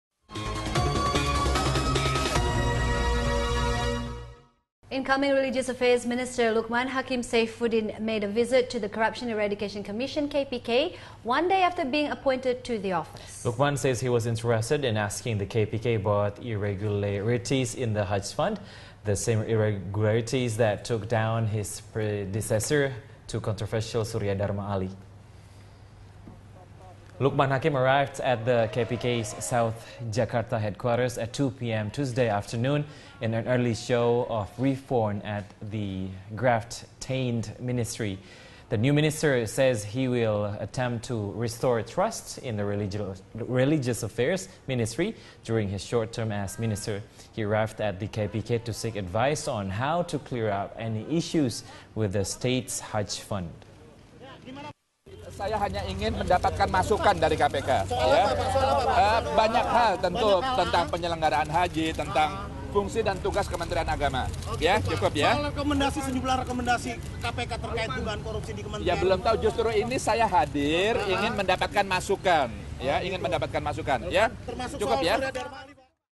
New Religious Affairs Minister Meets With KPK Over Hajj Fund Irregularities
Incoming Religious Affairs Minister Lukman Hakim Saifuddin made a visit to the Corruption Eradication Commission, KPK, on Tuesday, one day after being appointed to the office. Lukman says he was interested in asking the KPK about irregularities in the hajj fund — the same irregularities that took down his predecessor, the controversial Suryadharma Ali.